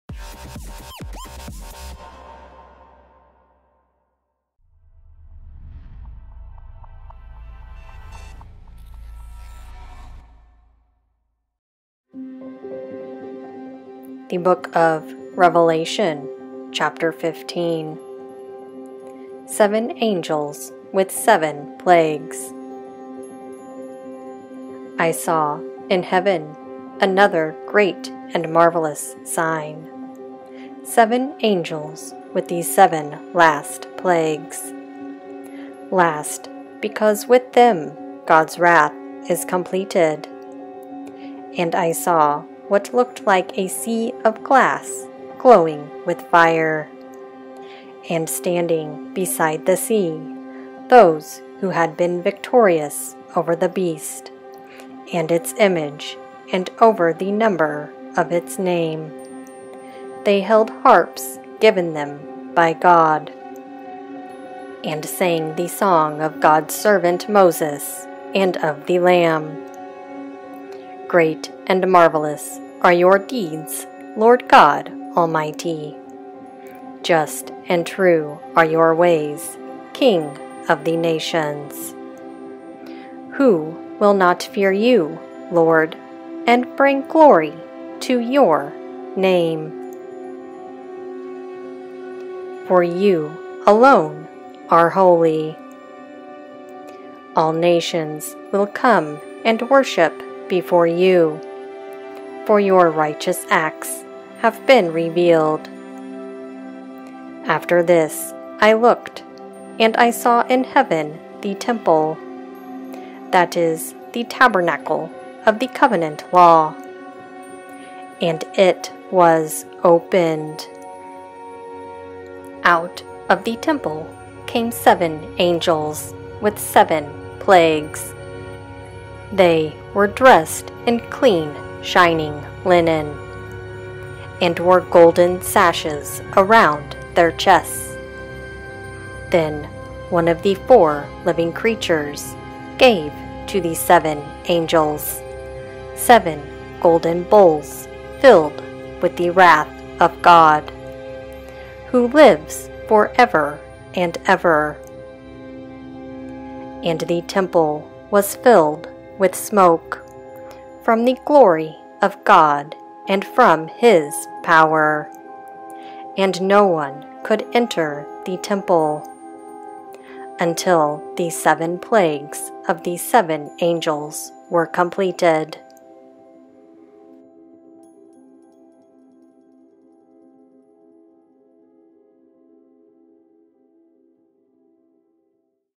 Revelation 15 | Discover The Last Plagues On Earth
Enjoy this short 3 minute video to unveil the seven bowls of God's wrath, the very last plagues brought to the Earth, as the angels come out of the Holy Tabernacle with the seven bowls.

Timestamp:
00:00 Intro
00:16 Revelation Chapter 15
00:23 Seven Angels with Seven Plagues
01:09 The Song of God's Servant Moses and The Lamb
01:56 Tabernacle of Covenant Law Opened
02:30 Seven Golden Bowls Filled With the Wrath of God